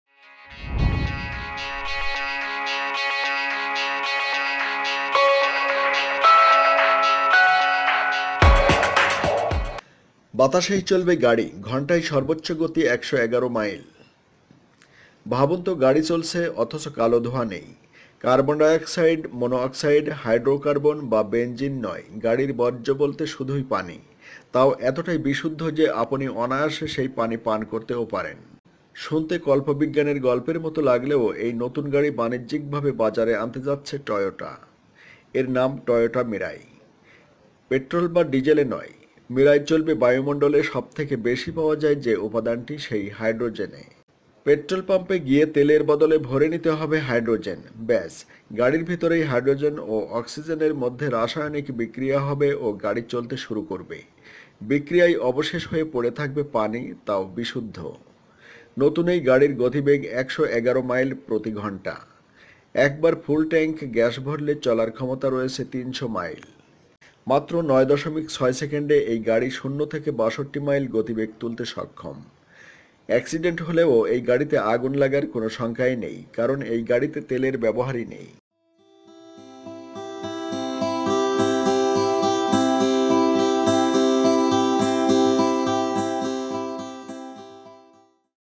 যে গাড়ি চলবে বাতাসে! ঘণ্টায় সর্বোচ্চ গতি ১১১ Mph
ভাবুন তো, গাড়ি চলছে, অথচ কালো ধোঁয়া নেই। কার্বন ডাই অক্সাইড, মনো অক্সাইড, হাইড্রো কার্বন বা বেঞ্জিন নয়, গাড়ির বর্জ্য বলতে শুধুই পানি। তাও এতটাই বিশুদ্ধ যে আপনি অনায়াসে পান করতে পারেন। শুনতে কল্প বিজ্ঞানের গল্পের মতো লাগলেও এই নতুন গাড়ি বাণিজ্যিকভাবে বাজারে আনতে যাচ্ছে টয়োটা। এর নাম টয়োটা মিরাই।